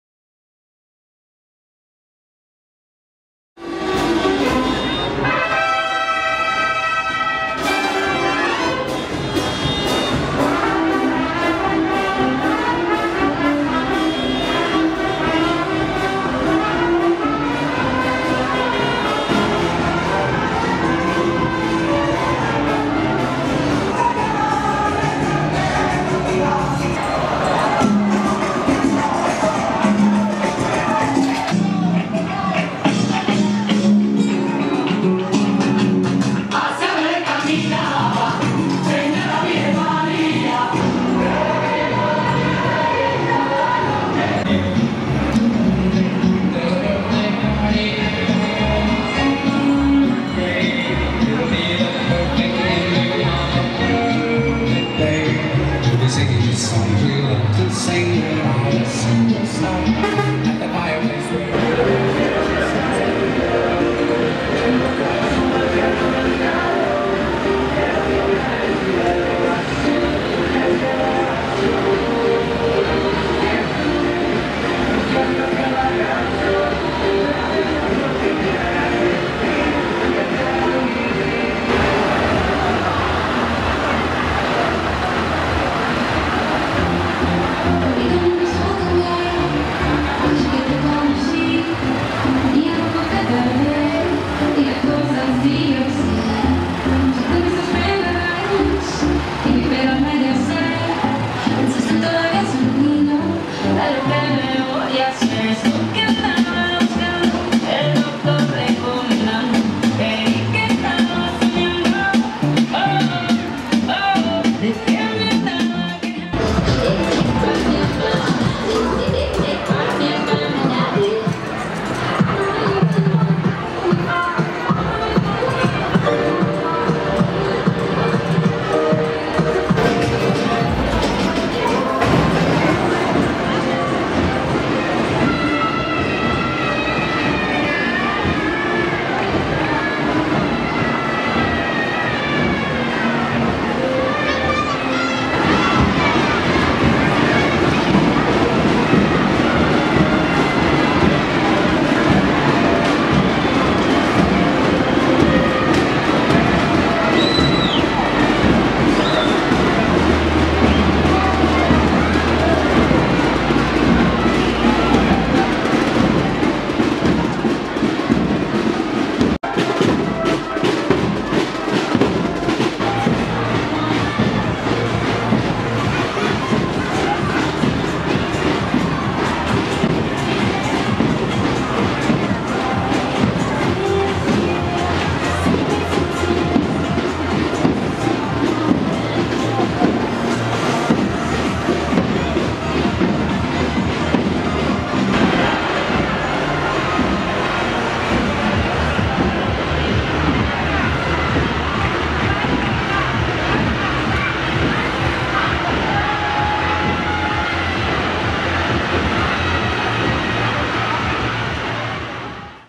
Cabalgata de Reyes 2018-Alcalá la Real
Resumen de la Cabalgata de Reyes 2018 en Alcalá la Real.